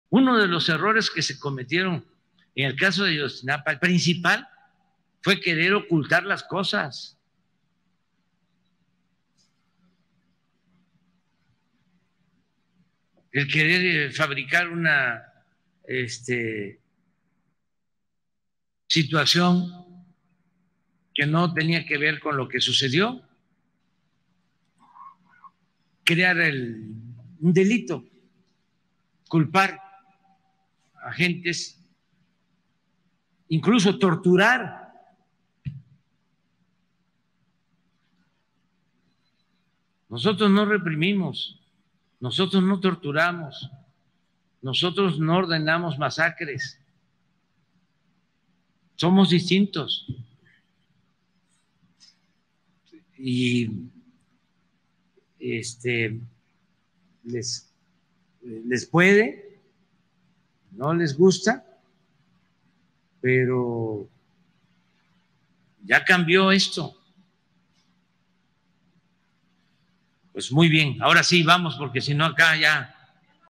AMLO: ERROR PRINCIPAL DE GOBIERNO EN CASO AYOTZINAPA FUE QUERER OCULTAR LAS COSAS
El presidente López Obrador señaló que el principal error del gobierno de Peña en el Caso Ayotzinapa fue querer ocultar las cosas, fabricar d3l1t0s, culpar a gente e incluso t0rtur4rl4. El mandatario reiteró que su gobierno no reprime, no t0rtur4, no ordena m4s4cres porque es distinto, y que eso les gusta a algunos, pero tienen que entender que esto ya cambió.